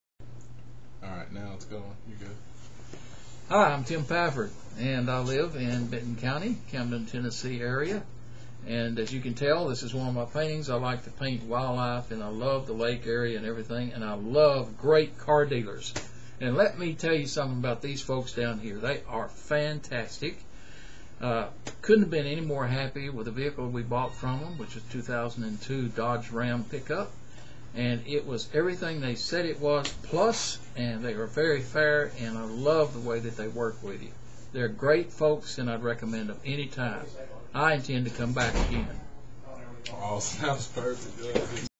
Another Great Gary Mathews of Jackson Customer Testimonial
Take a look at another Great Customer Testimonial with Mr. Pafford from the Nashville area! Mr. Pafford purchased a 2002 Dodge Ram 1500 from Internet Sales Manager Glenn Kasper. Also take a look at his wonderful painting if you want a great deal or painting like this please give us a call today at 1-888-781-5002 or ! Thanks